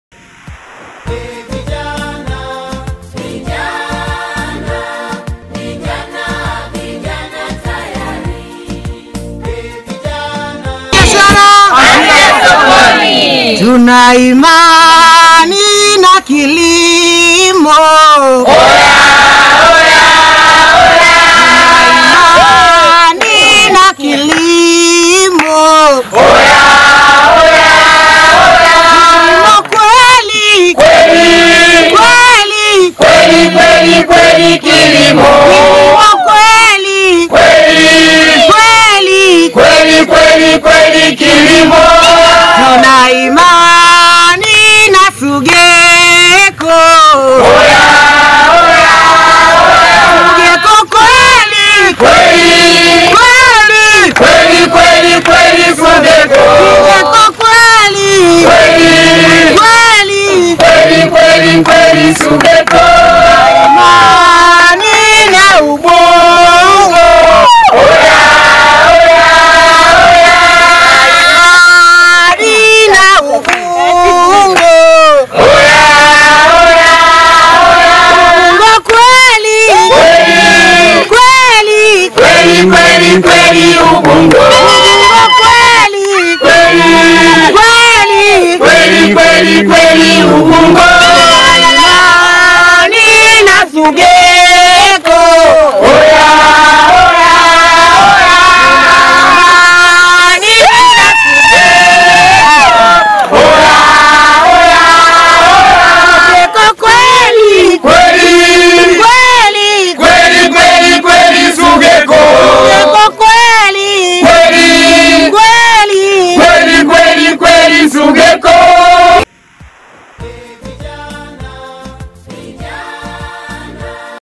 WANUFAIKA WA MAFUNZO UBUNGO
SUGECO kwa kushirikiana na Serikali yatekeleza mradi wa kilimo cha mazao ya mbogamboga kibiashara wa kuwafikia vijana zaidi ya 2000.